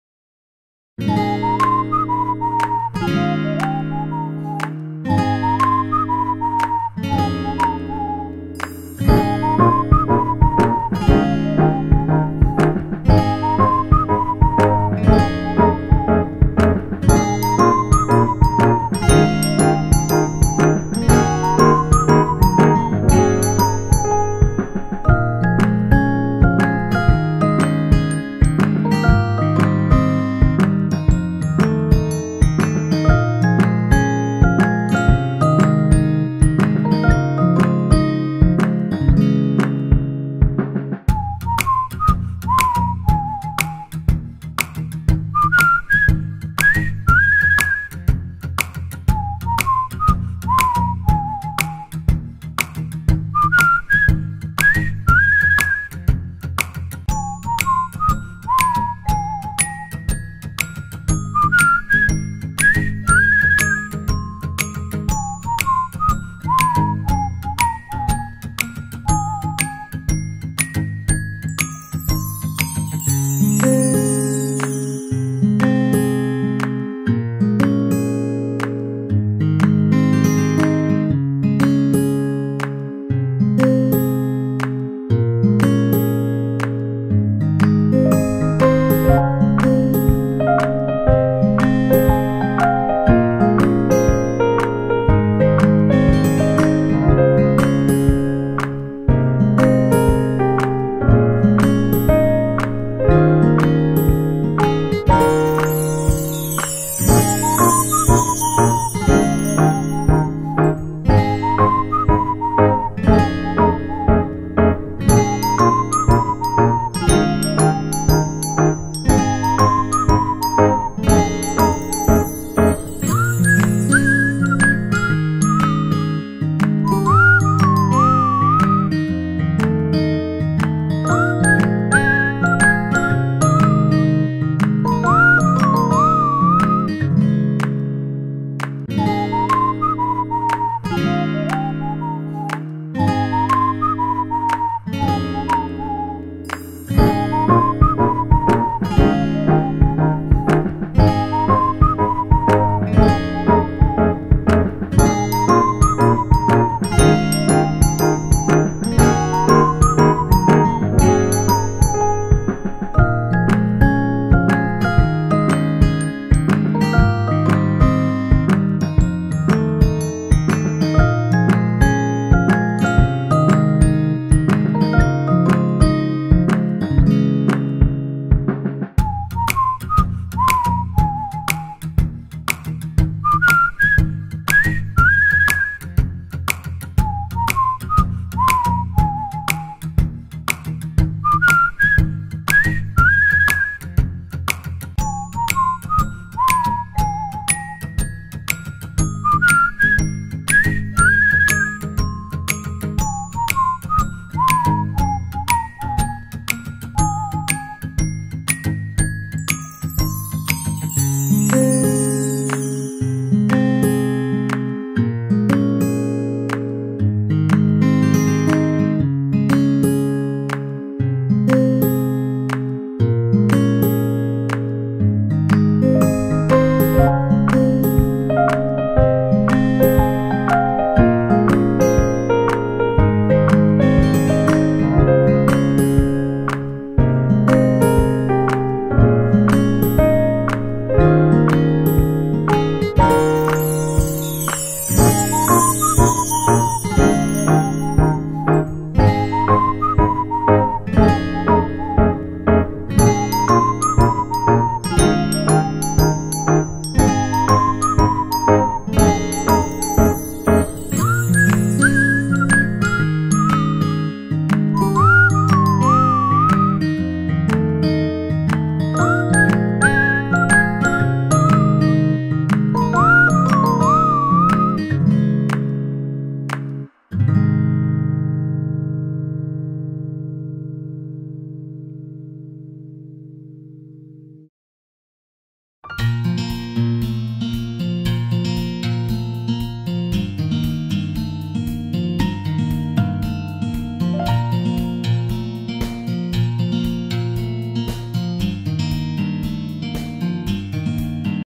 鉄筆＆色鉛筆を使ってのんびりひたすら猫の絵を描く動画
メルカリ、SUZURIで販売も行っているのでよかったら見て行ってください♪